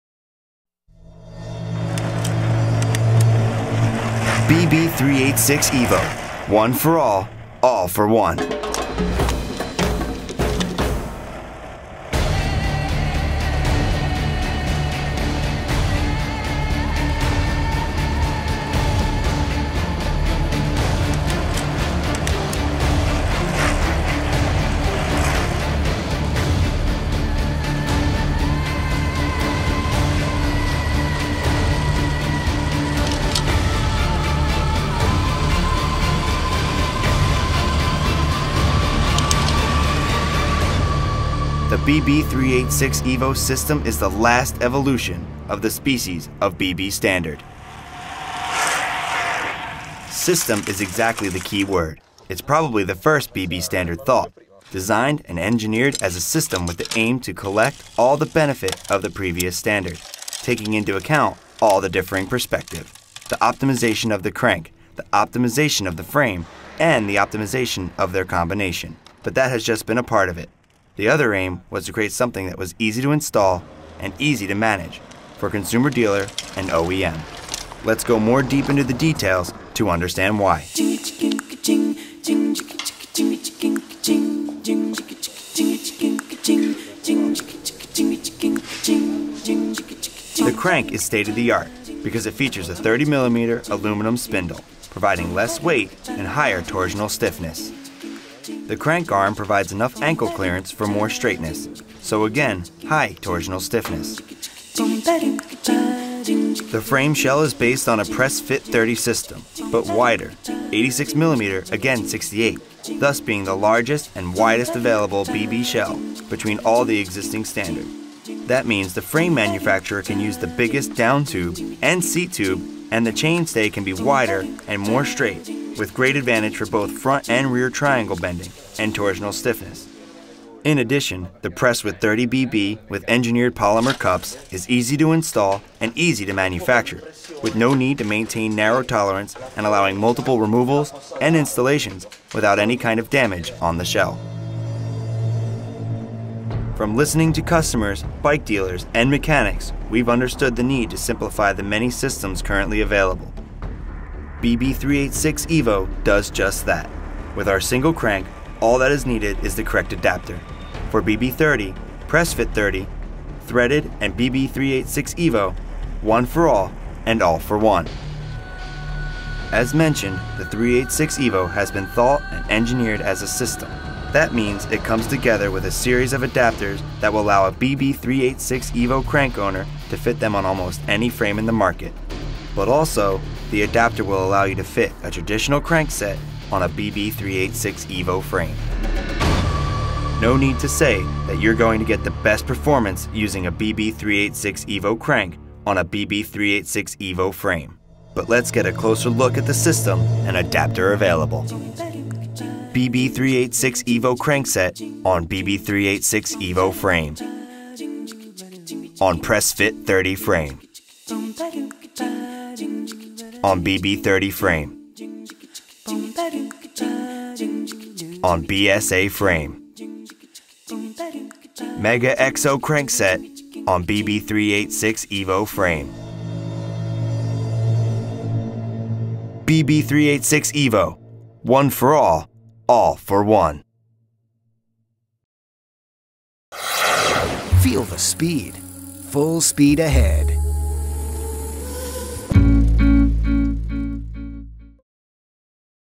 BB386 EVO Road Cranksets Introduction - FSA Road
A full explanation of the new standard BB386 Evo Bottom Bracket System, how it works and it's compatibility.

Full Speed Ahead is a world class bicycle components manufacturer, for road bikes, mountain bikes and triathlon.